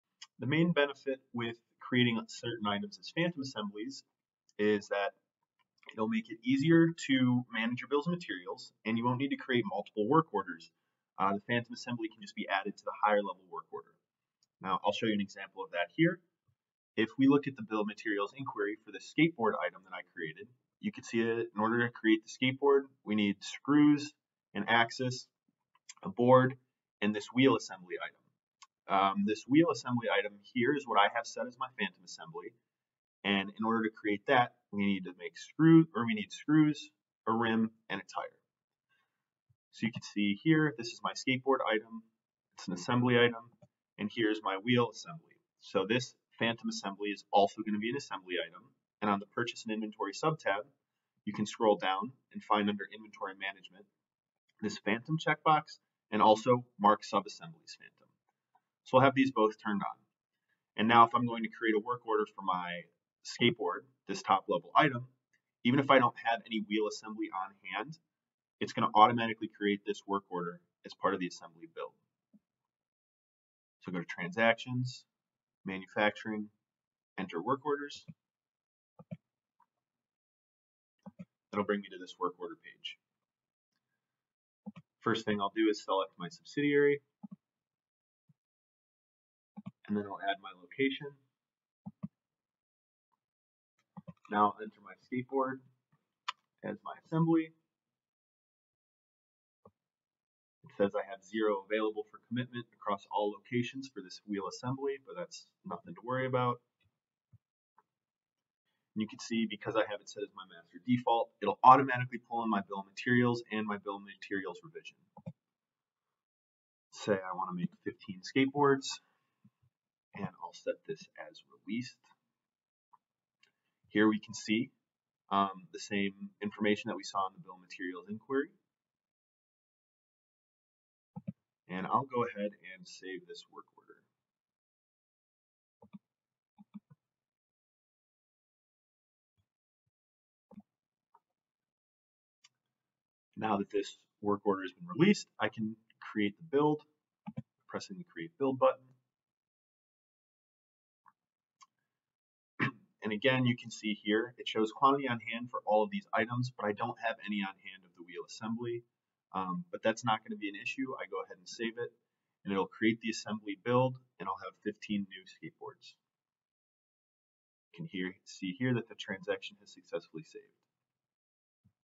NetSuite: Phantom Assembly Demo
This demo explains the phantom assembly feature in NetSuite.